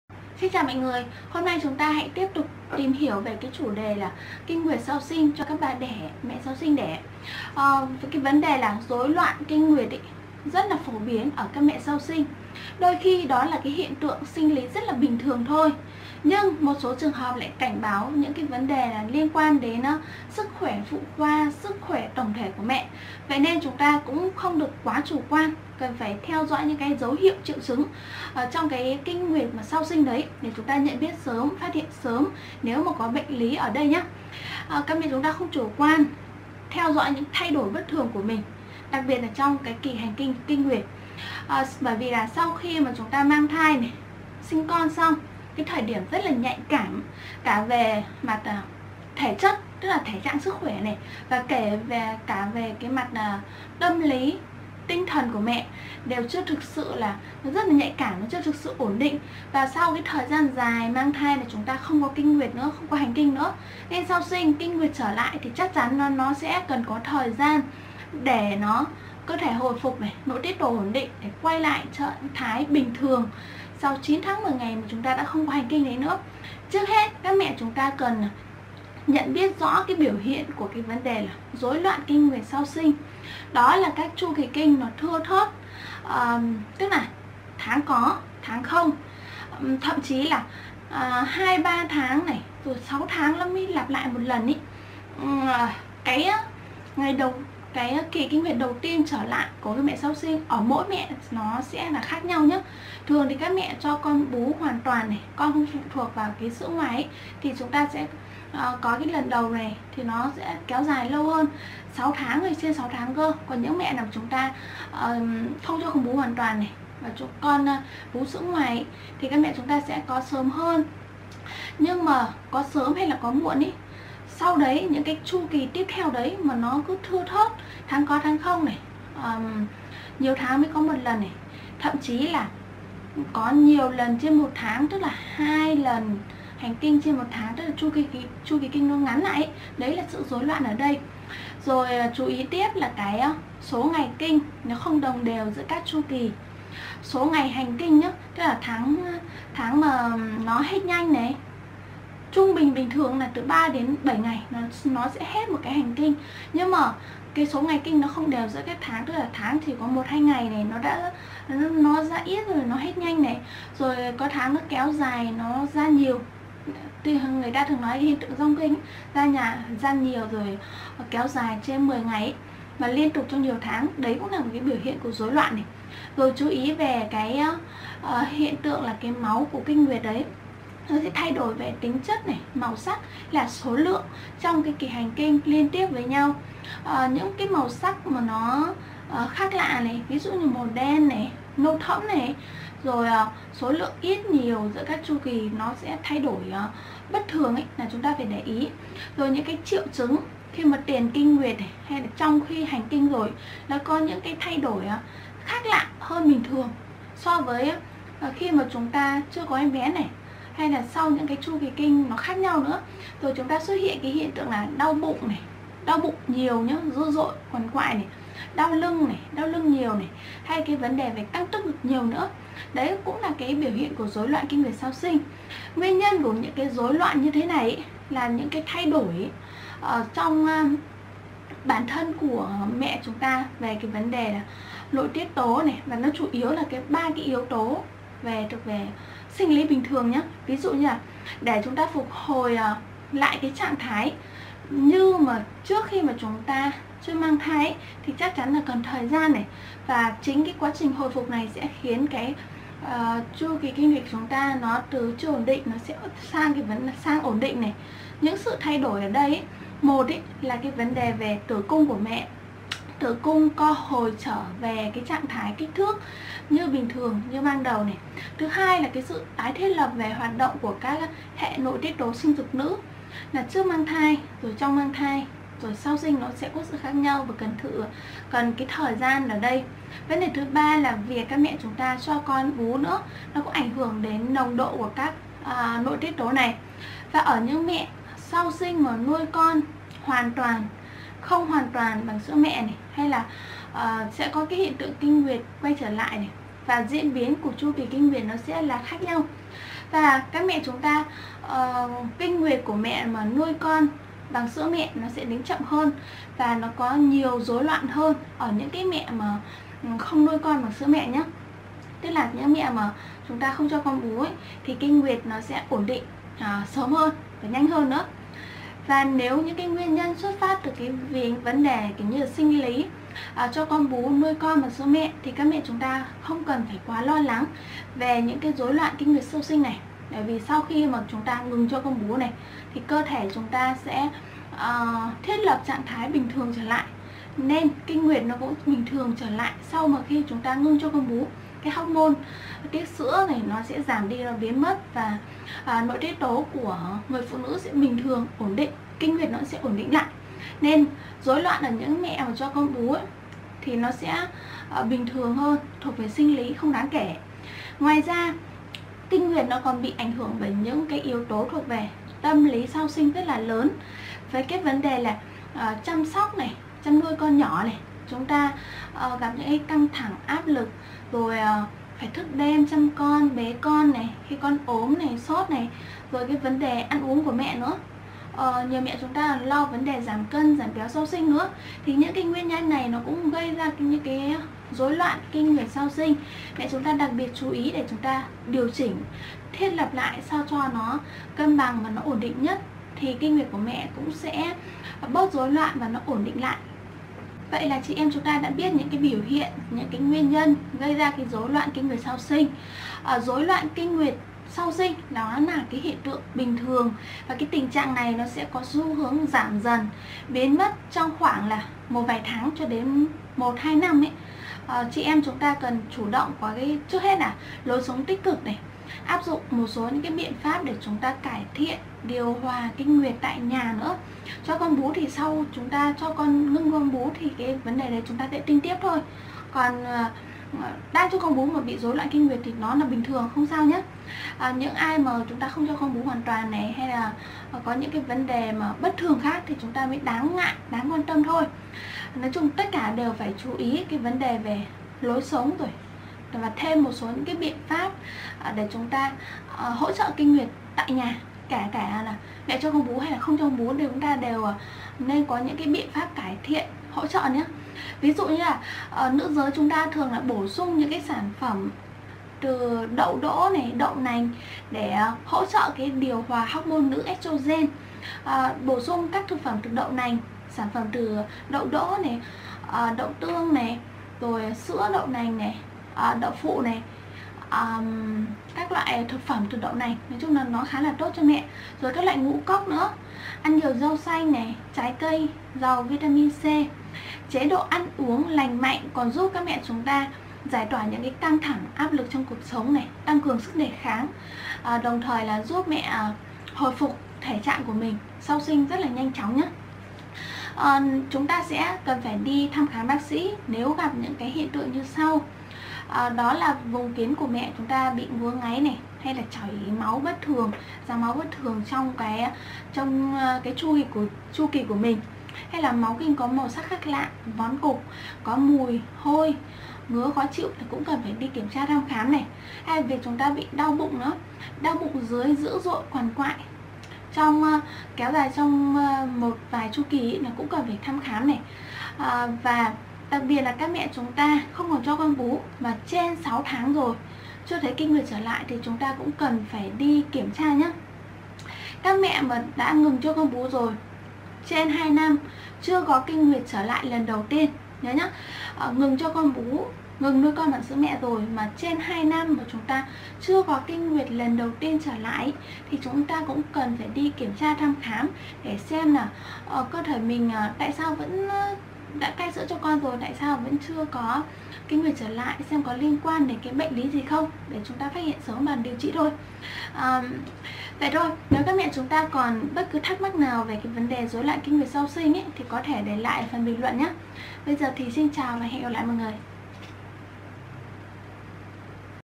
Bà đẻ phụ nữ sau sinh bị Rối loạn kinh nguyệt: Dấu hiệu, nguyên nhân và cách khắc phục
Rối loạn kinh nguyệt sau sinh là hiện tượng các bà mẹ không có kinh nguyệt trở lại sau khi đã sinh con hoặc chu kỳ diễn ra bất thường, lúc có lúc không, kinh ra ồ ạt hoặc rất ít, thay đổi màu sắc... Cùng tìm hiểu rõ hơn các biểu hiện, nguyên nhân và cách điều trị kinh nguyệt bất thường cho bà đẻ sau sinh nhé.